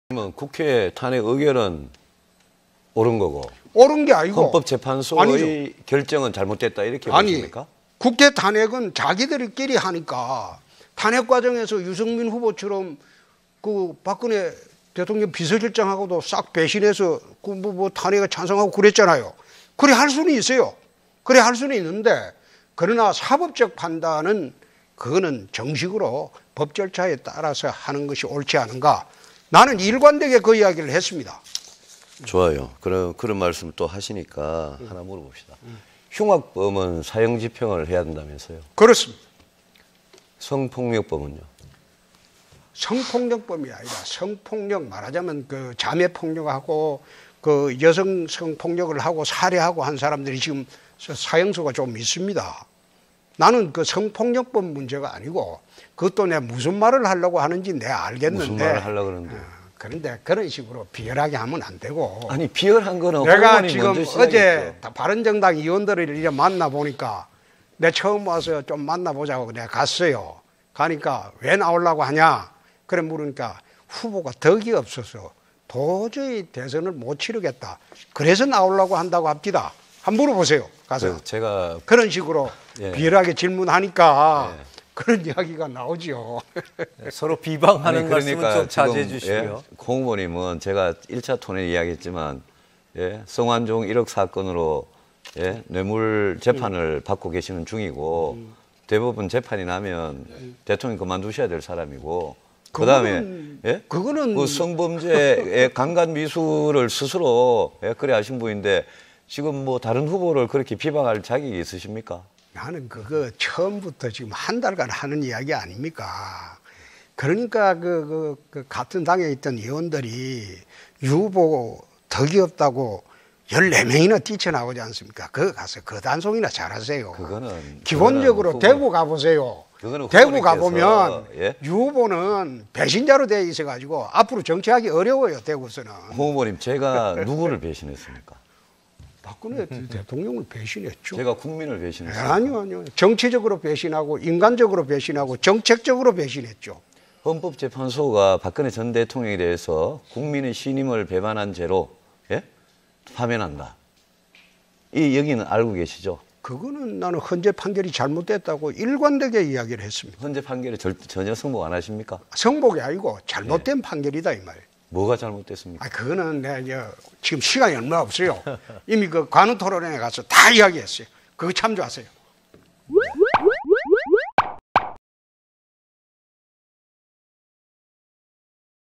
유승민vs홍준표 사형제도 [170502 마지막 토론회 공방]
5월 2일 진행된 마지막 토론회에서 사형제도를 주제로 공방을 벌이는 유승민과 홍준표